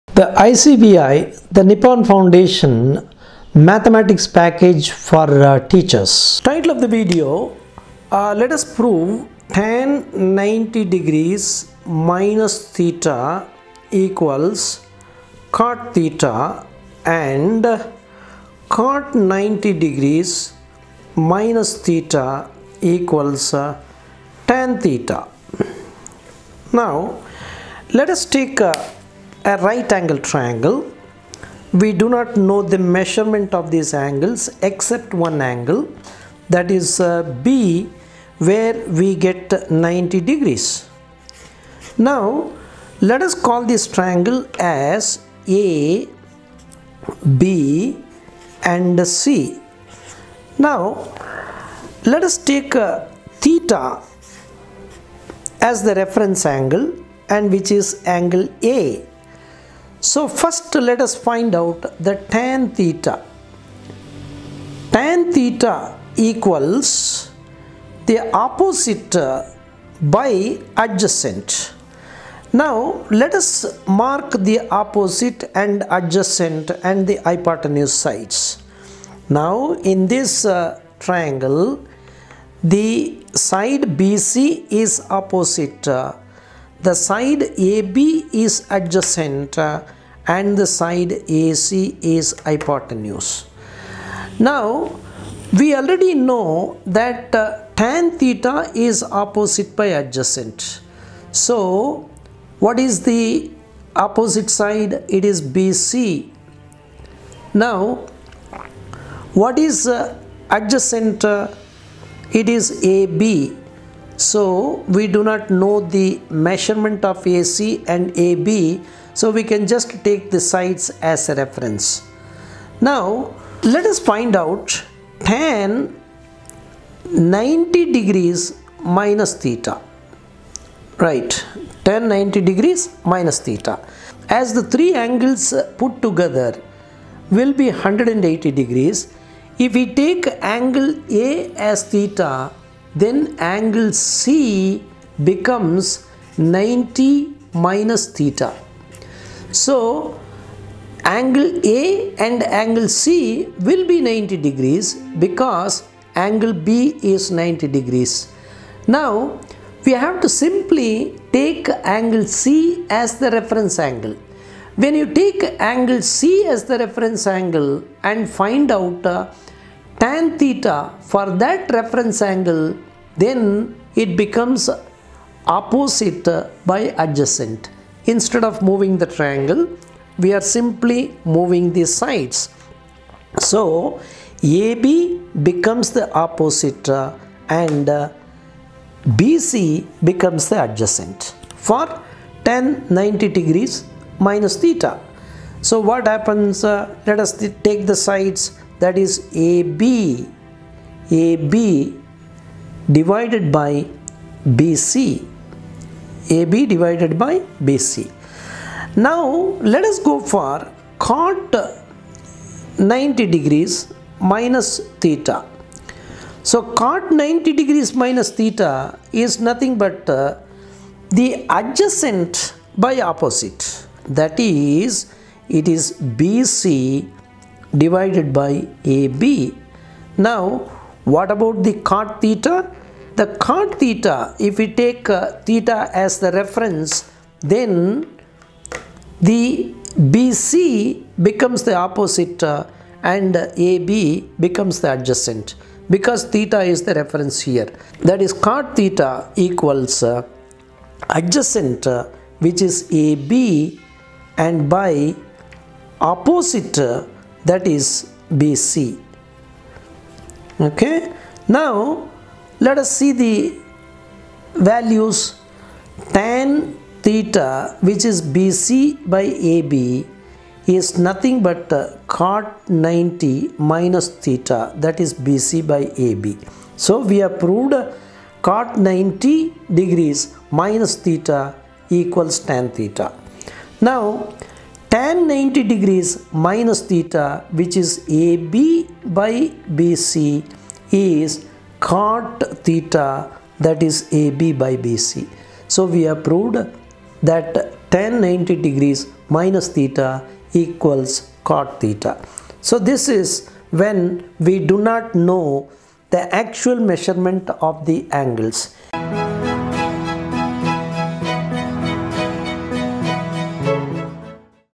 Tan (90- Ө) = Cot Ө and Cot (90- Ө) = Tan Ө
Finding out the trigonometric ratios of complementary angles is also an area of study in trigonometry.  The video explains relationship between the Tangent of the complementary angle Theta and Cotangent Theta and Vice-Versa.